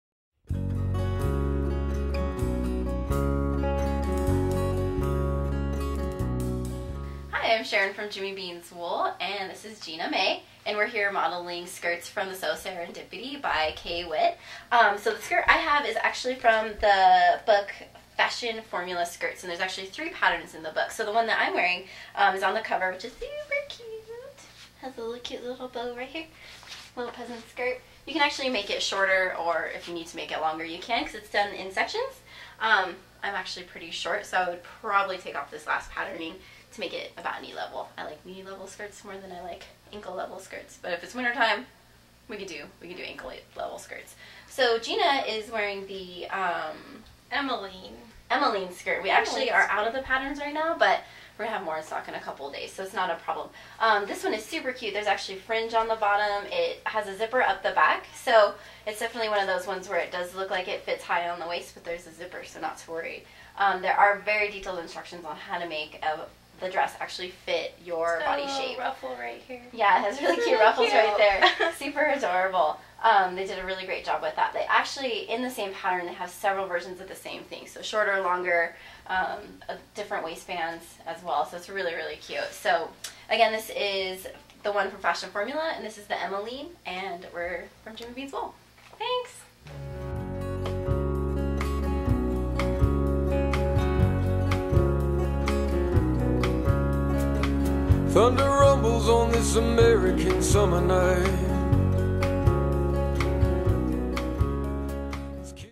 Serendipity Studio Sewing Patterns Fashion Formula Skirts Booklet and The Emaline Skirt Review
Gina from Jimmy Beans Wool reviews Serendipity Studio Sewing Patterns Fashion Formula Skirts Booklet Sewing and Quilting Patterns and Serendipity Studio Sewing Patterns The Emaline Skirt Sewing and Quilting Patterns.  Jimmy Beans Wool is your LYS... online!